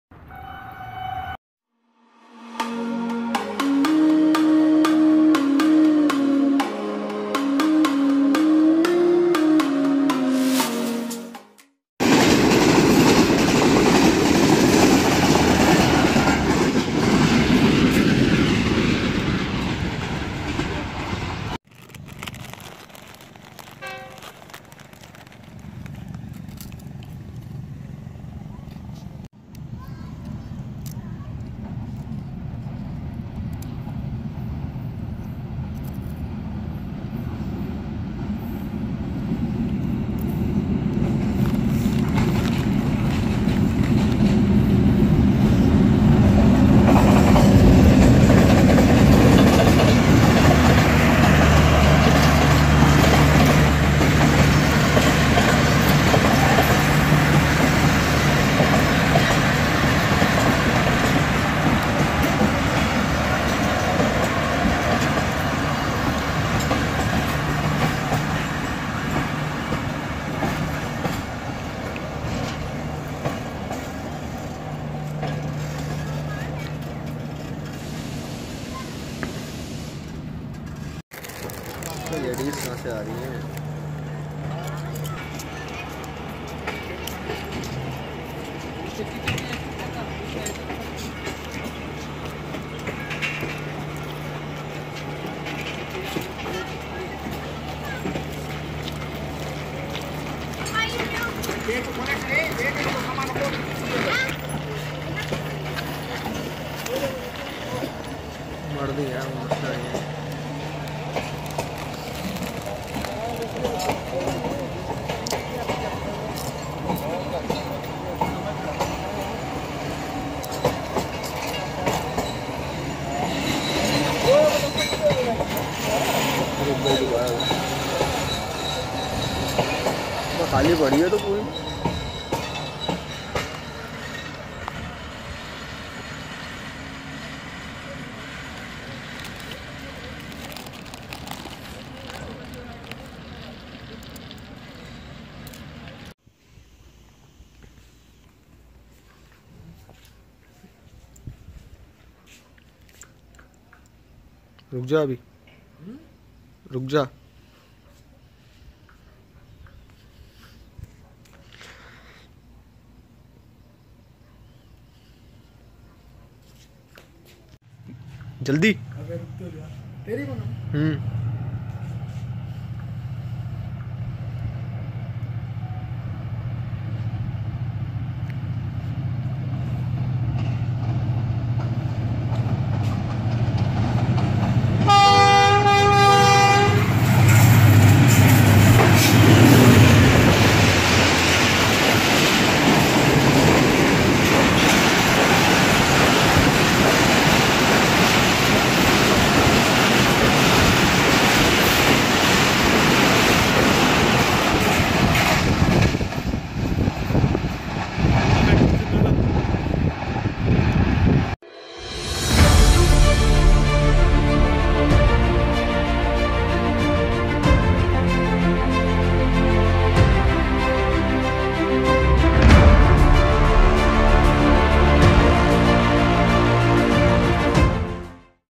Train in Night: Capturing ALLAMA IQBAL & AKBER EXPRESS from PANO AQIL CANTT | Pakistan Railways
ALLAMA IQBAL is the regular Train service between Karachi to Sialkot. the assign number is 9 up from Karachi to Sialkot. Similarly, 10 dn from Sialkot to Karachi. The train consist of totally Economy Class Coaches.

TRAIN ROUTE:
Karachi - Sialkot (via Hyderabad, Rohri, Jahania, Sahiwal, Lahore)

AKBER EXPRESS is the regular train service between Quetta to Lahore. The assign number is 23 up from Quetta to Lahore. Similarly, 24 dn from Lahore to Quetta. The train consist of Economy and AC Standard Class Coaches.

TRAIN ROUTE:
Quetta-Lahore (via Rohri, Jhanian, Faisalabad)